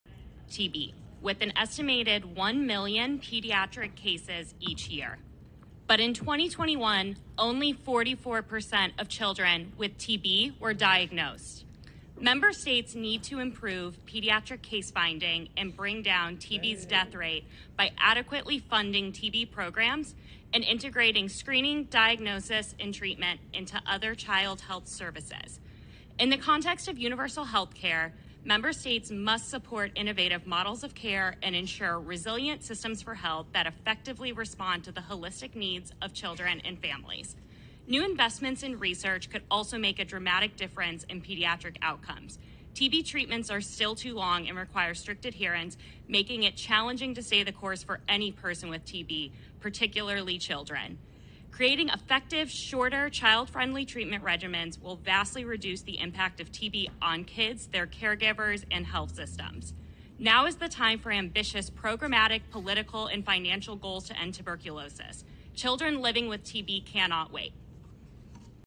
UNHLM TB Statement
EGPAF's Officer of Public Policy and Advocacy, Cosette Audi, addresses the delegates at the United Nations High Level Meeting on Tuberculosis.

Transcript:
Children bear a heavy burden of TB, with an estimated 1 million pediatric cases each year.

But in 2021, only 44% of children with TB were diagnosed.

Member States need to improve pediatric case finding and bring down TB’s death rate by adequately funding TB programs, and integrating screening, diagnosis and treatment into other child health services. In the context of universal health care, Member States must support innovative models of care and ensure resilient systems for health that effectively respond to the holistic needs of children and families.

New investments in research could also make a dramatic difference in pediatric outcomes. TB treatments are still too long and require strict adherence, making it challenging to stay the course for any person with TB, particularly children. Creating effective, shorter child-friendly treatment regimens will vastly reduce the impact of TB on kids, their caregivers and health systems. Children must be included in the development and implementation of next generation vaccines as well.

Now is the time for ambitious programmatic, political, and financial goals to end tuberculosis. Children living with TB cannot wait.